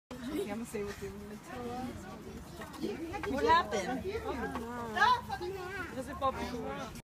Catering a birthday party!
Espresso bar & crepe station catering for events of ANY size & type.